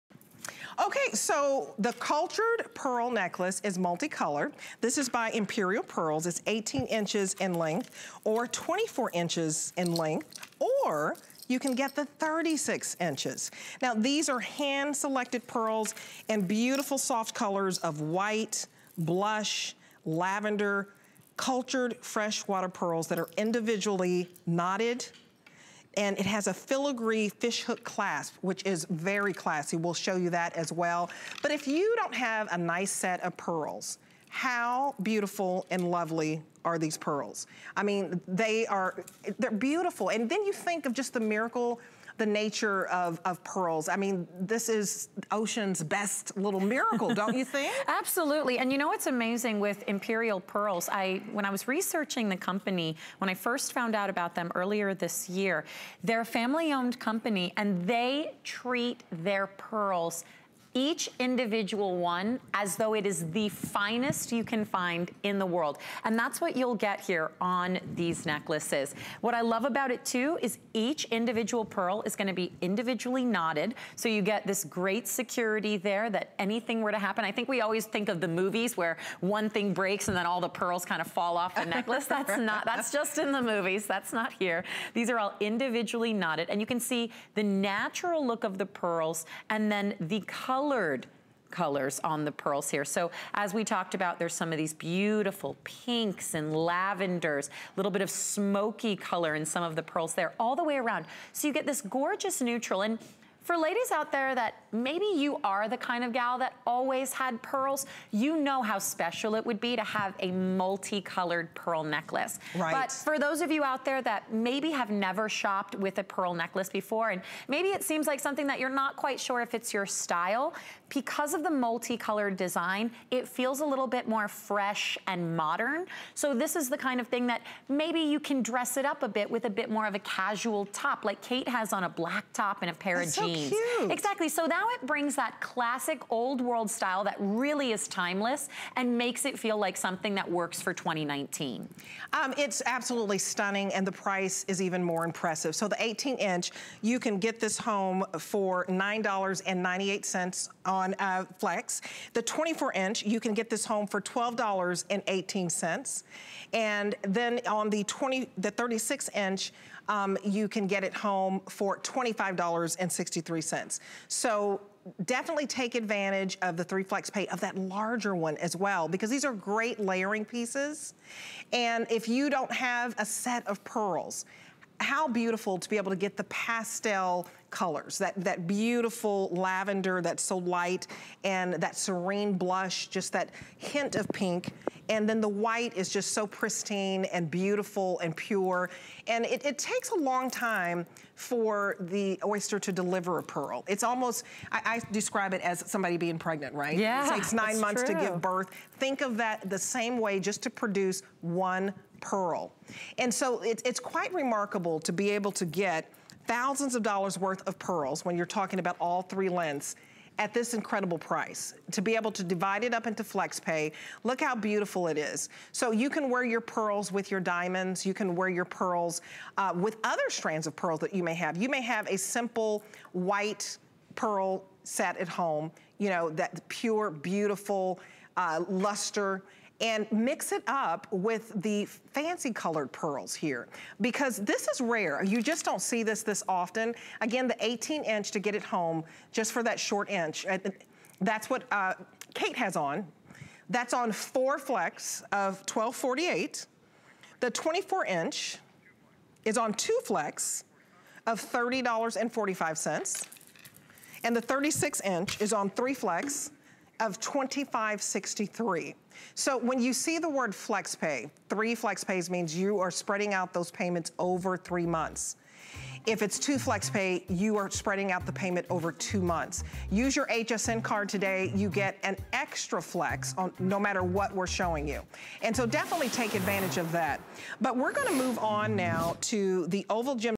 Imperial Pearls 8.39.3mm Multicolor Cultured Pearl 18" N...
Imperial Pearls 8.39.3mm Multicolor Cultured Pearl 18" Necklace
Glistening hues of sunrise glow along the length of this strand. A lovely look for any time of year and especially spring and summerthe necklace features pearls in soft colors that occur naturally during the culturing process. It's a special find for any finejewelry collection.

Design Information

                Necklace made with white, blush and lavender cultured freshwater pearls
                Individually knotted strand